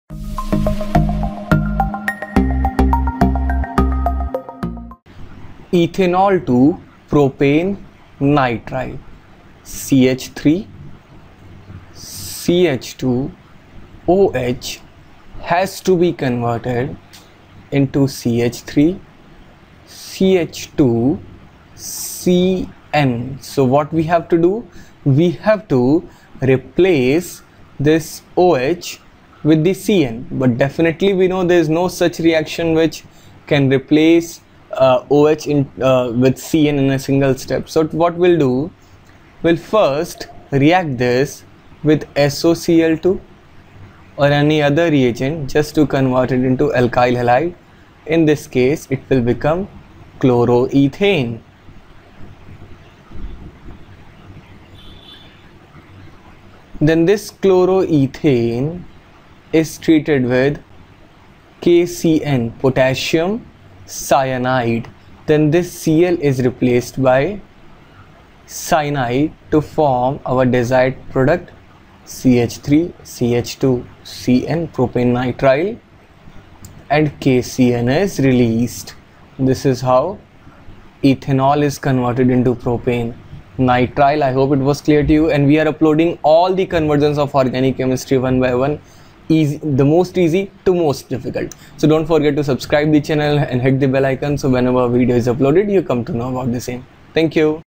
Ethanol to Propane nitrile | Organic chemistry conversions for JEE, NEET & CBSE
Convert ethanol to propane nitrile conversions of organic chemistry class 12. how to do conversions haoalkanes, alkyl halide, chemistry concept. chemistryconcept.com